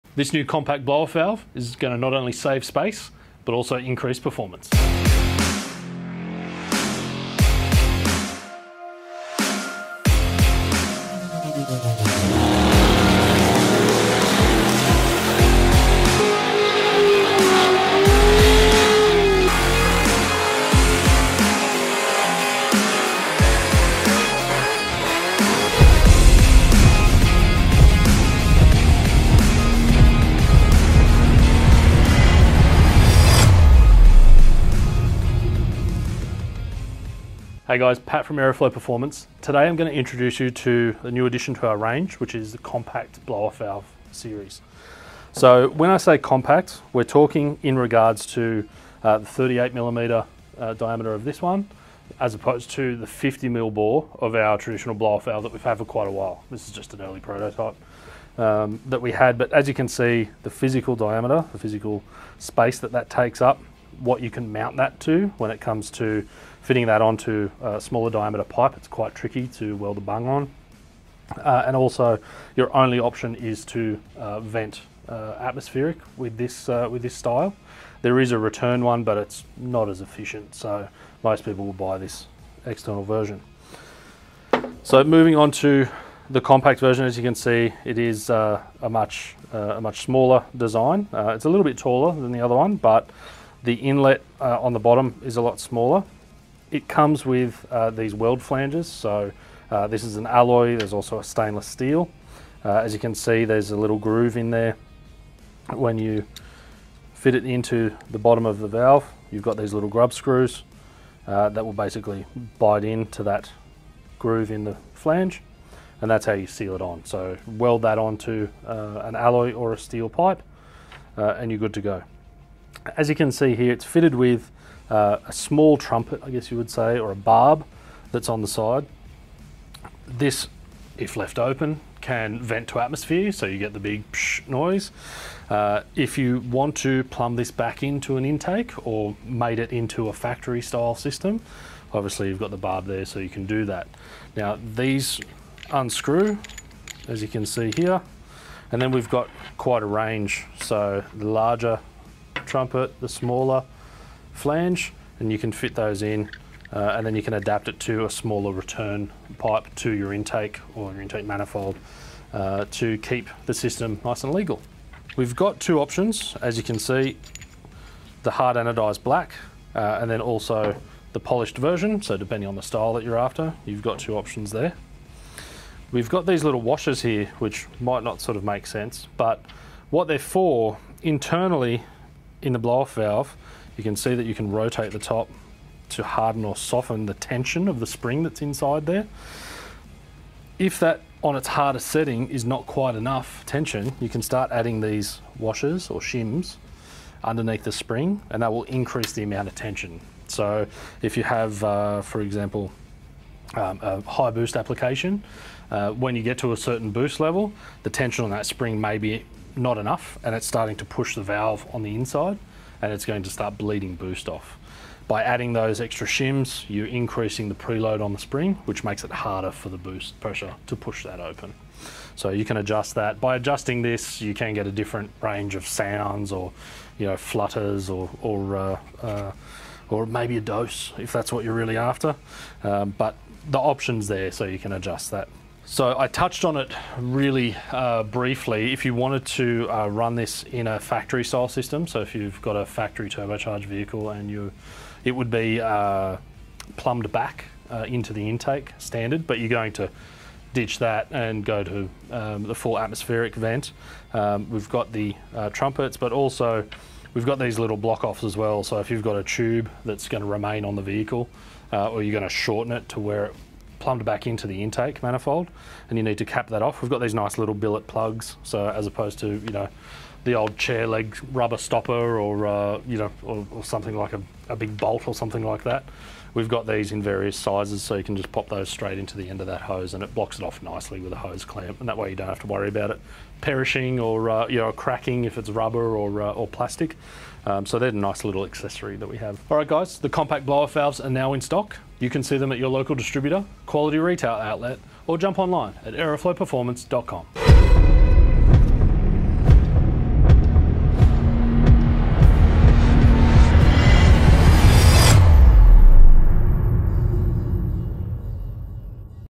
Compact Blow Off Valve
The Aeroflow Performance universal compact vent to atmosphere blow off valve is designed to keep your turbo shaft speed at a maximum which is greatly reduced in speed by changing gears or backing off the throttle and the air found in your intercooler pipes bouncing back off the throttle blade and surging against the compressor wheel causing it to slow and feel lagging when the next gear is pulled. For use on turbocharged and supercharged applications. The body components are CNC-machined from 6061 aluminium alloy with brass internals. The adjustable top body can be screwed in each direction to provide increased or decreased spring tension with added shims to internally adjust this for more spring tension. An alloy and stainless weld on pipe are included in each kit. These weld on pipes lock onto the bottom of BOV via a lock ring with grub screws to secure into place and seals with a Viton O-ring. The outlet screw in barb included in the kit is 32mm barb. Various other sizes are sold separately under our plumb back barb adapter range (20mm up to 38mm).